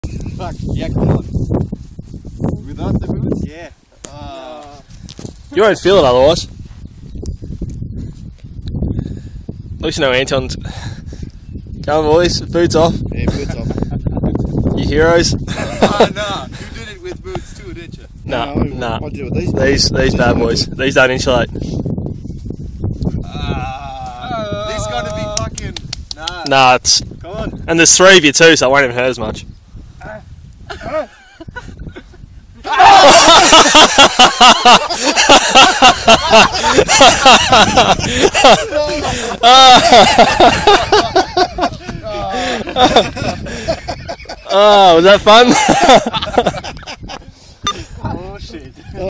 Backpacker touching electric fence - Christmas tree farm
Working on the christmas tree farm